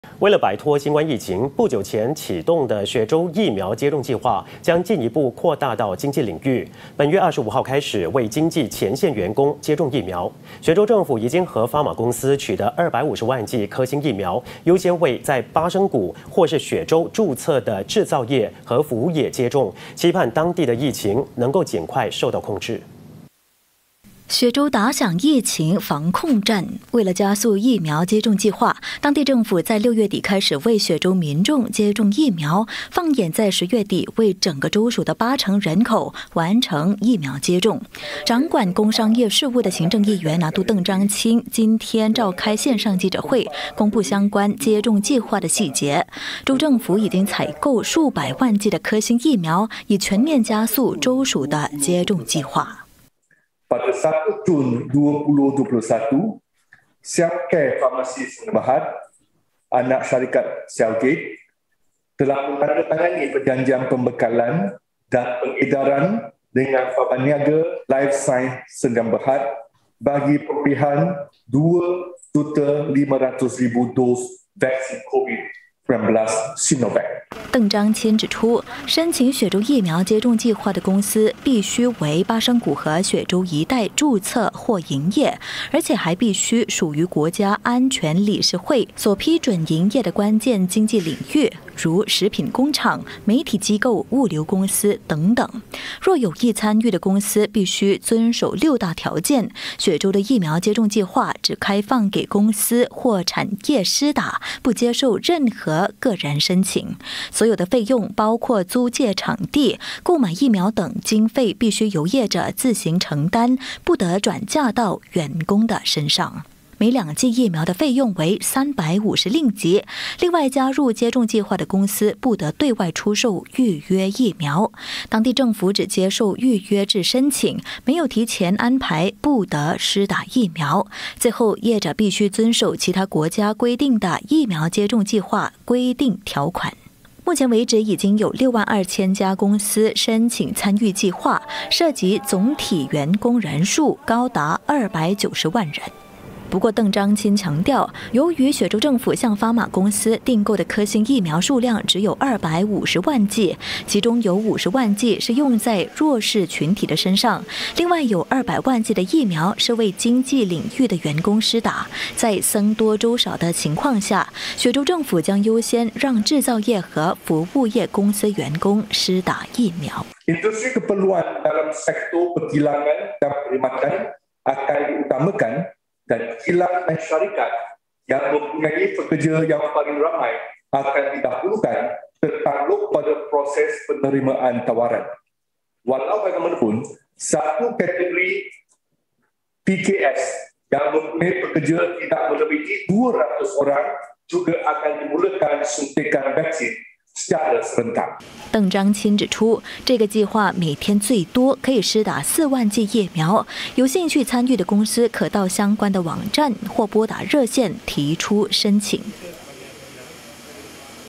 【雪州扩大疫苗接种范围 725开放企业领域打疫苗】
为了摆脱新冠疫情，不久前启动的雪州疫苗接种计划将进一步扩大到经济领域，本月25号开始为经济前线员工接种疫苗。

雪州政府已经和发马公司取得250万剂科兴疫苗，优先为在巴生谷或雪州注册的制造业和服务业接种，期盼当地的疫情能够尽快受到控制。